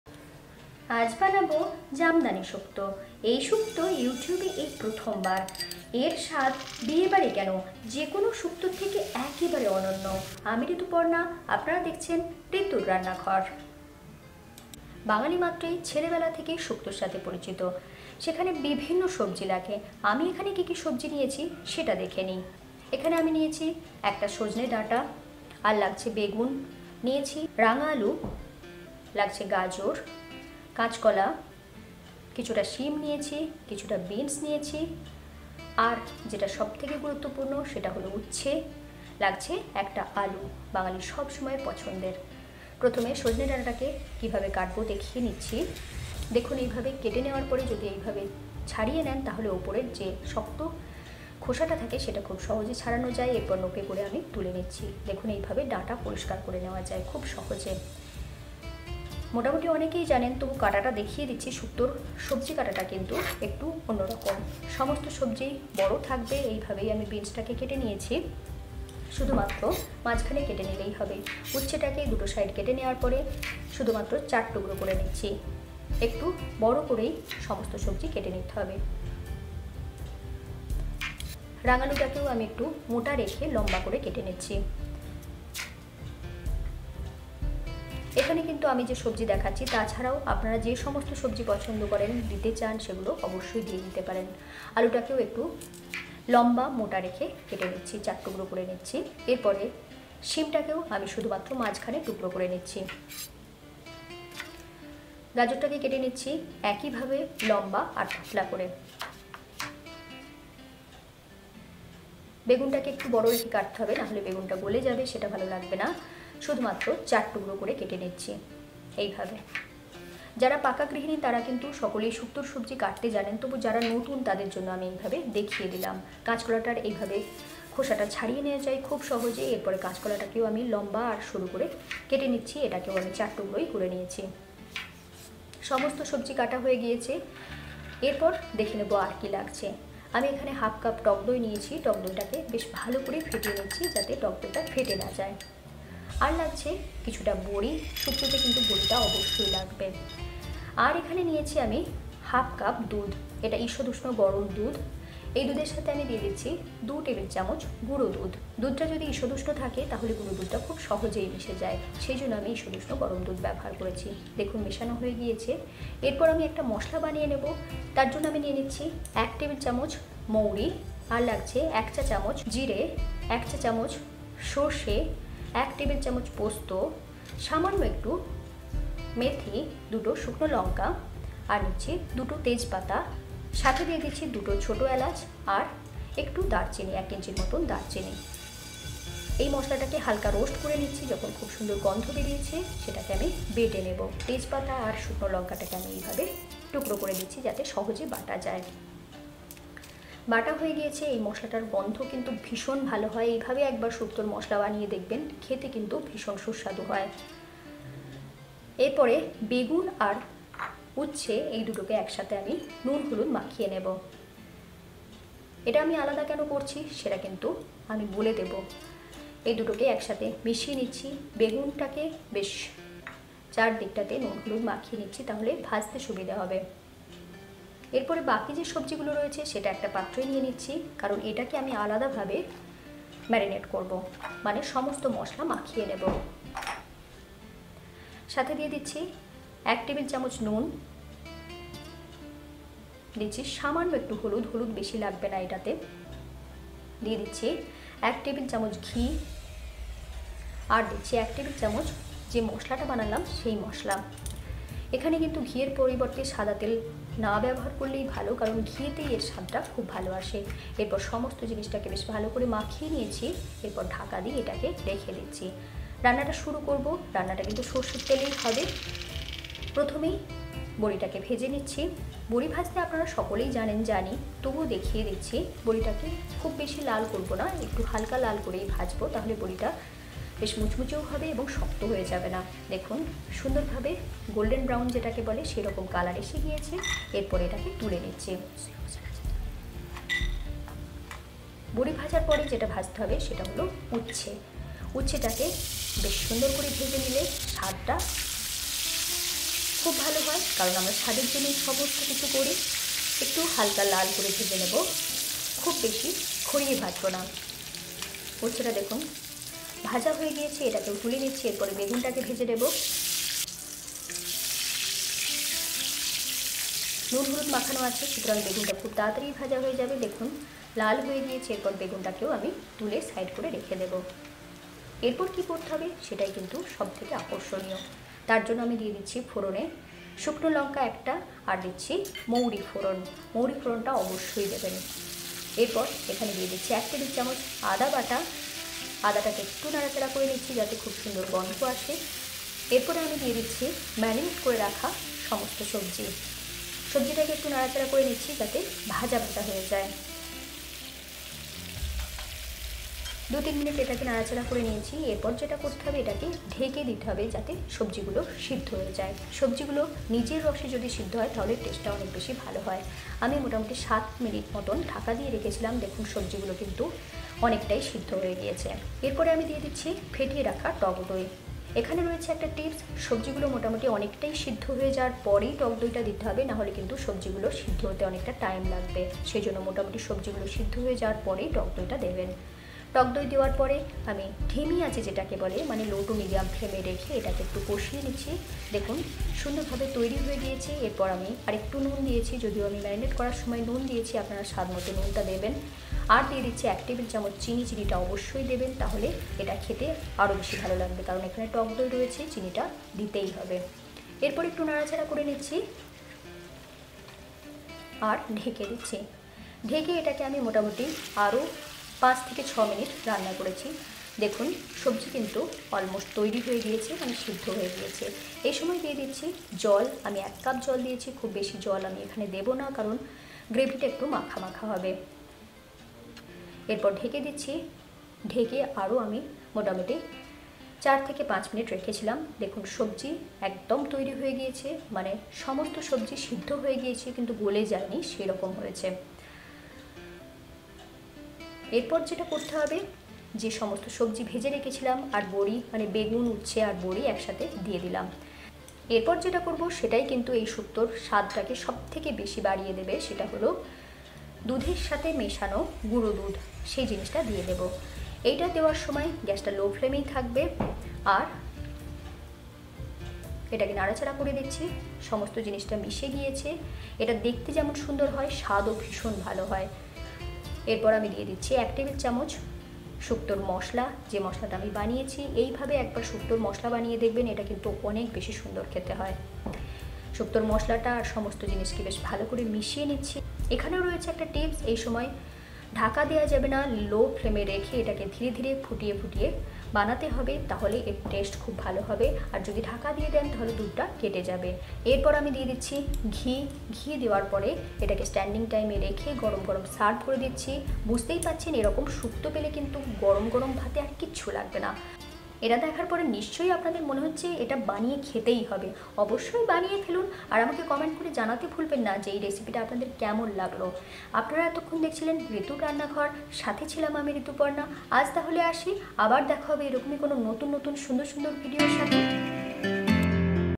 সিক্রেট মশলা সহ জামদানি শুক্তোর কাছে অনুষ্ঠান বাড়ির শুক্তো ও ফিকে | Unique Bengali Shukto Recipe
Ritur Rannaghar presents a unique Bengali Shukto Recipe which is a evergreen recipe in Bengali cuisine . The jamdani shukto recipe shared in this video is a unique recipe which taste heavenly.

শুক্তো রেসিপি, অনুষ্ঠান বাড়ির শুক্তো, Shukto Recipe, জামদানি শুক্তো, Brinjal Recipe in Bengali, Ritur Rannaghar, best bengali recipe, Niramish recipe,